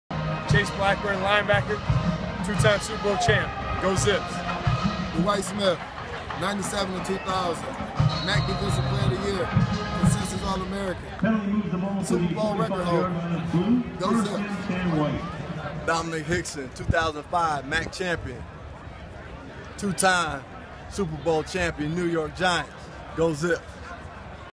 Inside Zips Football: 5-time Superbowl Champions
Akron Zips football alumni Domenik Hixon, Dwight Smith & Chase Blackburn have combined to win 5 Superbowls over their NFL careers. among their many lists of accomplishments!